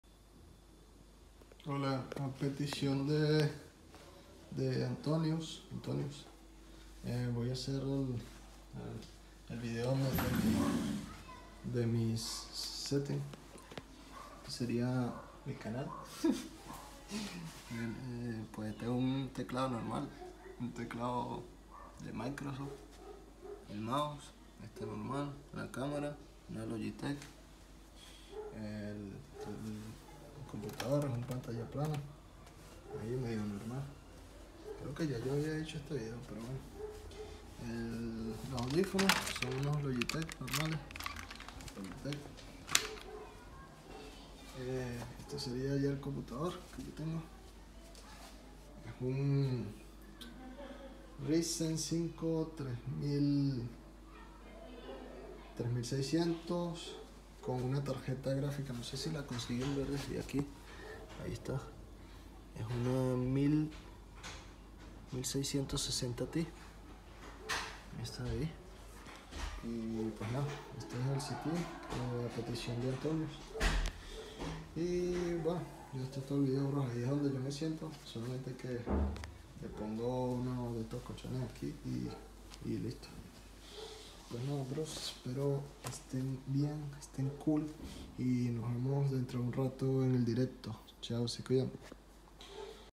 MI COMPUTADORA ANDRESKOLM
Les muestro mi computadora donde juego among us😆